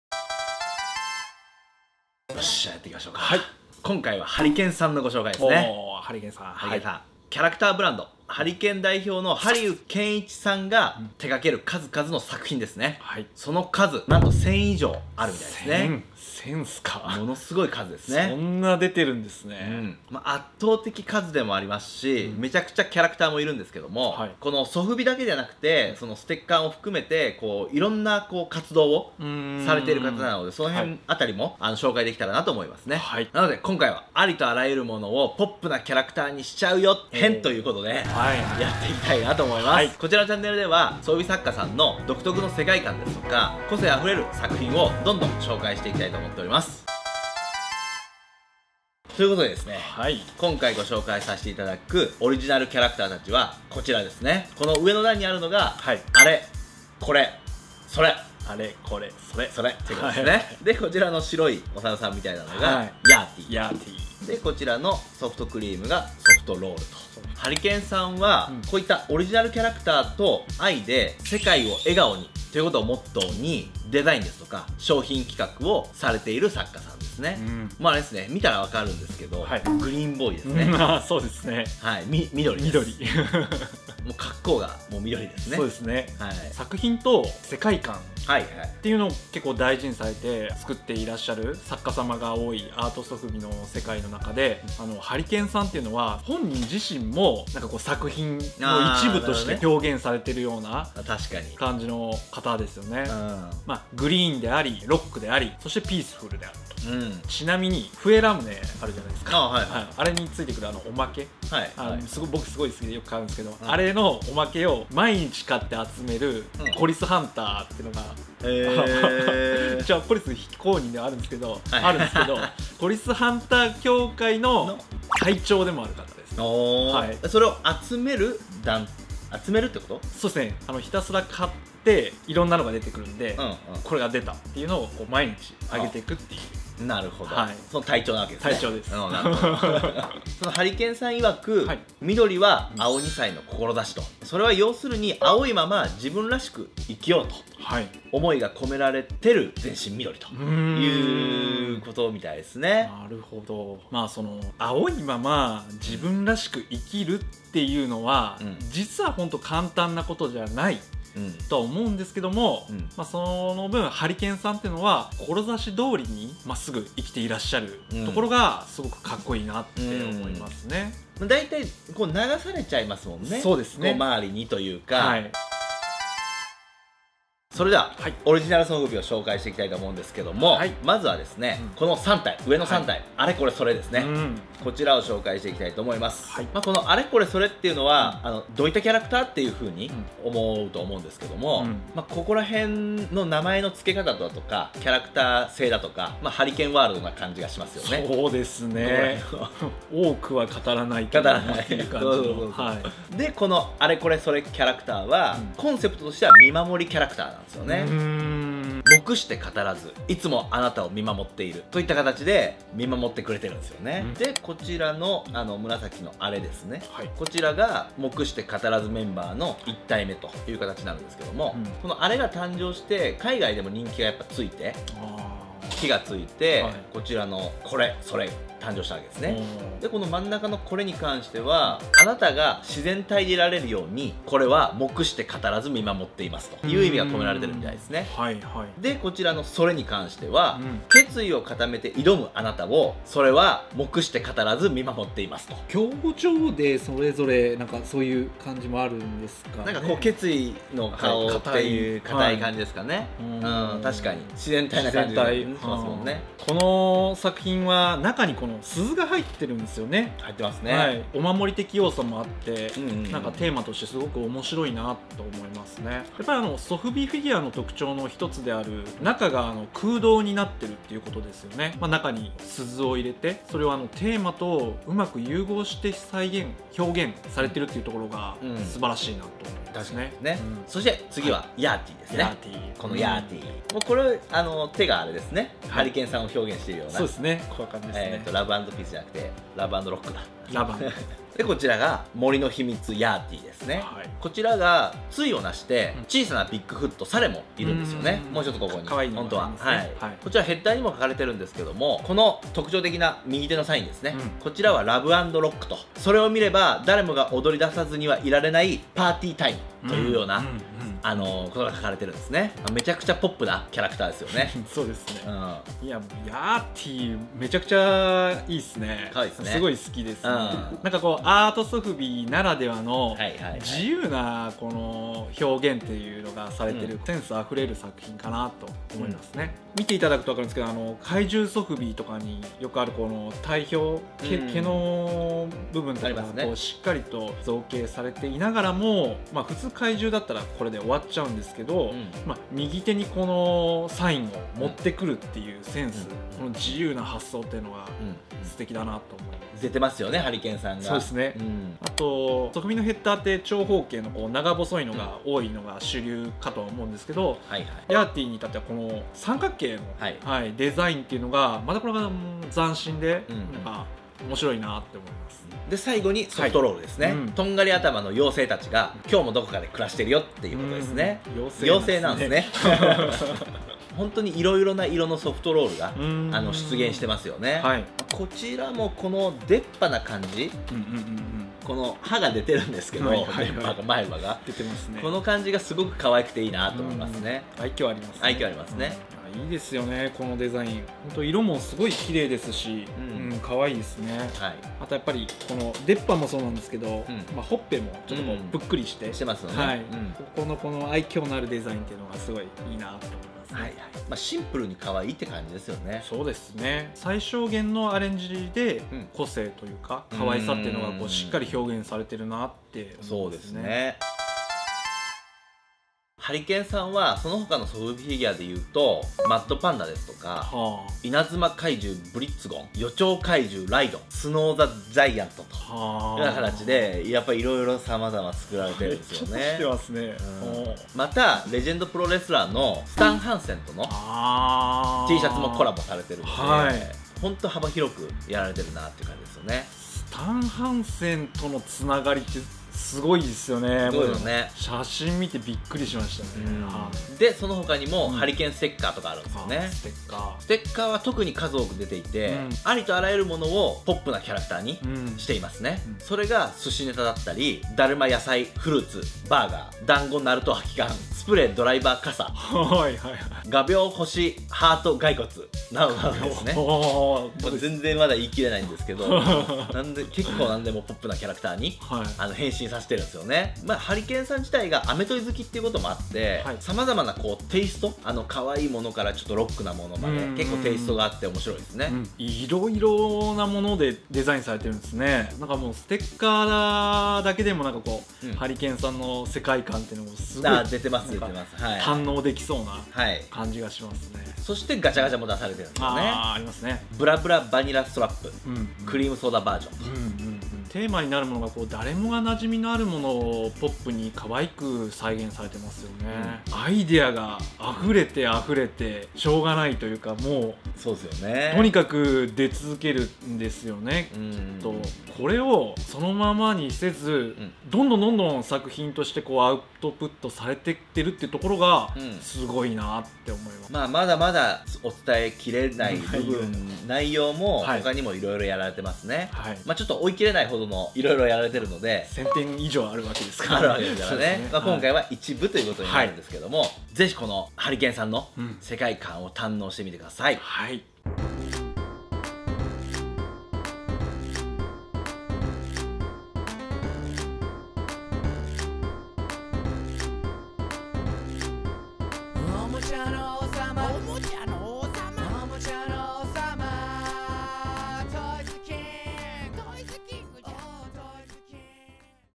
HARIKENワールド！グリーンボーイでお馴染みのHARIKENさんのオリジナルキャラクター！キャラクターブランド。ありとあらゆるものをポップなキャラへ。
HARIKENさんは『オリジナルキャラクターと愛で世界を笑顔に』をモットーにデザイン／商品企画で活動されている作家さんです。

【 SNS 】

【 販売サイト 】

【 買取サイト 】

▼ その他